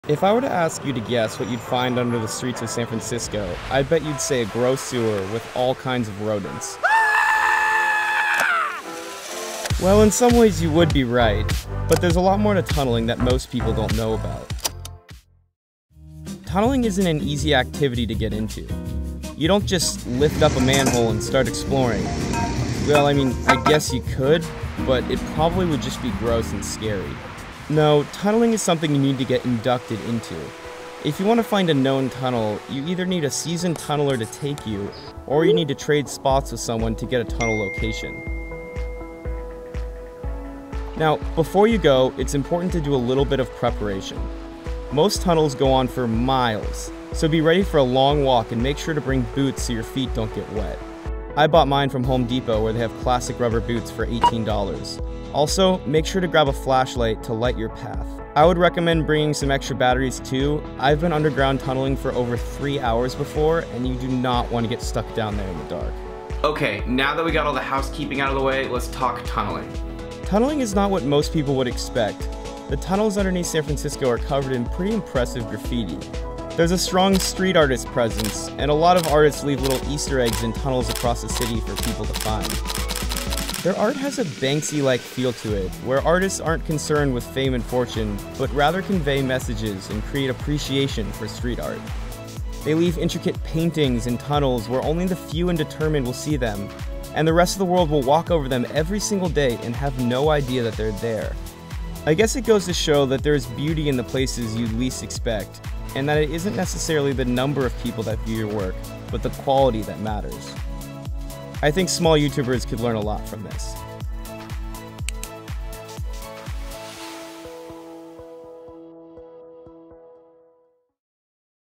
Tunnels beneath San Francisco are NOT what you would expect
The tunnels beneath San Francisco are probably NOT what you would expect. If you are fortunate enough to find the hidden tunnels there is a crazy path to walk along with all kinds of incredible graffiti.

New video next Monday!!